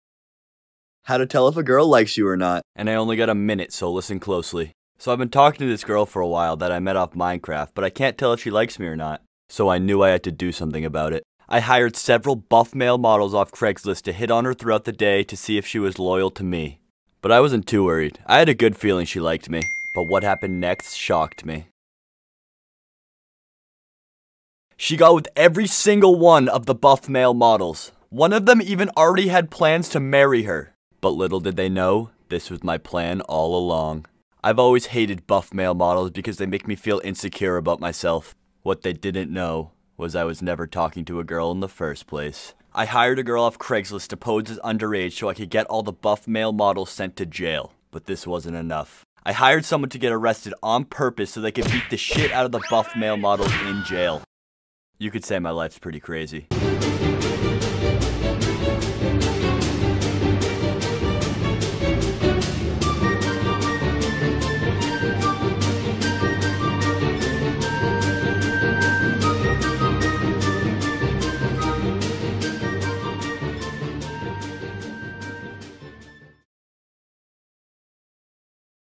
How To Tell If A Girl Likes You
How to tell if a girl likes you tiktok
hires buff male models to get in her DMs to see if she is loyal
was never talking to a girl in the first place
guy tricks male models
tik toks